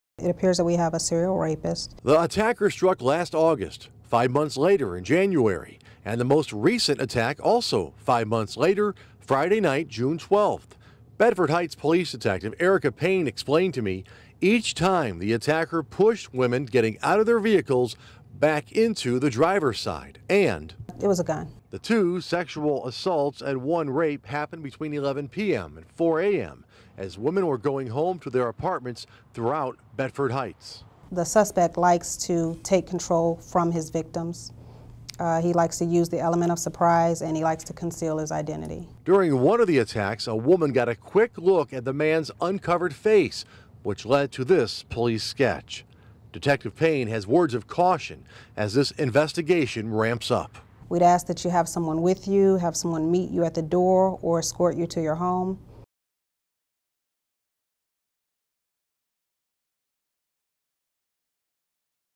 Bedford Hts. serial rapist update
Bedford Hts. police are seaching for a serial rapist attacking women near apartment buildings.